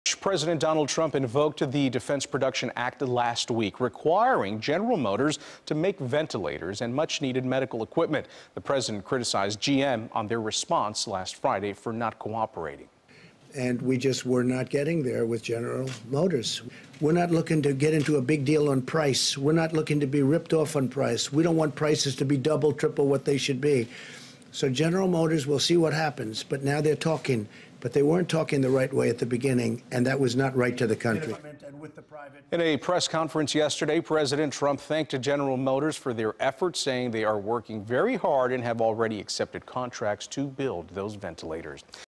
President Trump Invokes Defense Production Act Against General Motors
President Donald Trump invoked the Defense Production Act last week, requiring General Motors to make ventilators and much needed medical equipment.